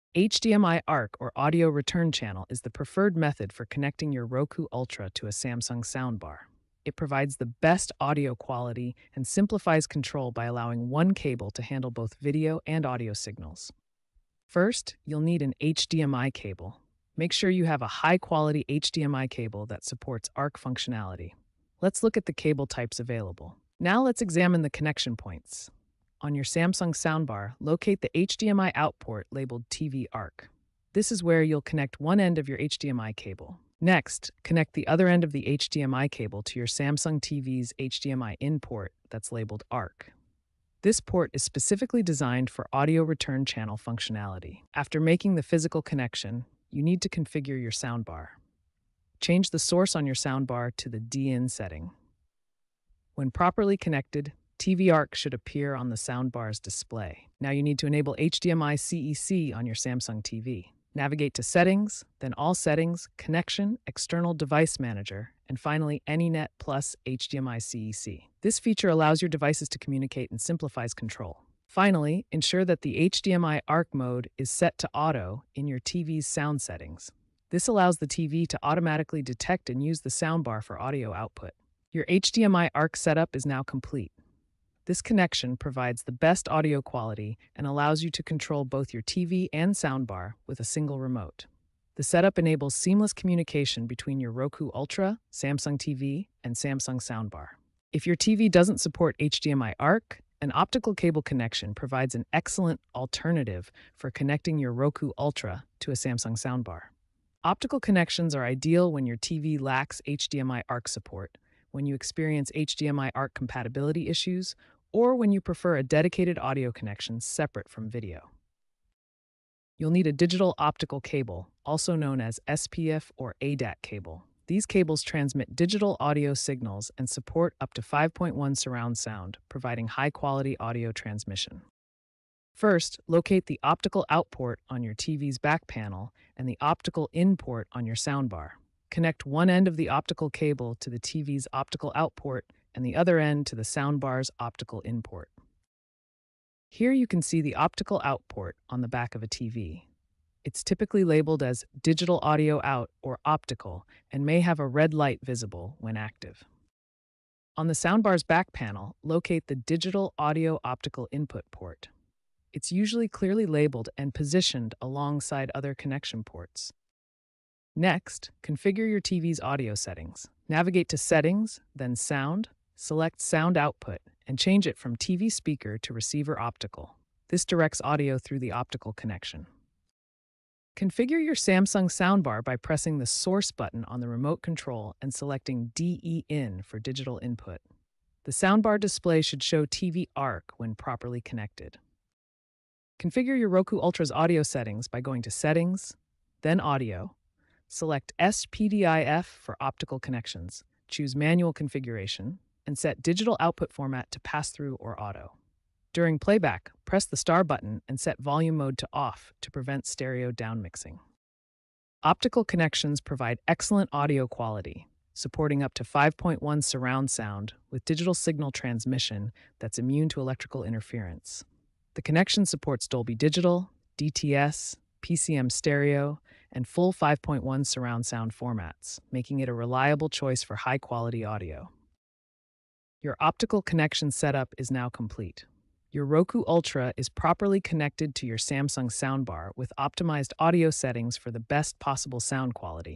How to Connect Roku Ultra to Samsung Soundbar – HDMI ARC & Optical Setup
In this comprehensive tutorial, we guide you through the process of connecting your Roku Ultra to a Samsung Soundbar using both HDMI ARC and Optical setups. Whether you're looking to enhance your home theater experience or simply want to enjoy superior sound quality, this video covers all the necessary steps to ensure a seamless connection. We will discuss the advantages of each method, provide troubleshooting tips, and demonstrate how to configure your devices for optimal performance. Join us as we make your entertainment setup easier and more enjoyable!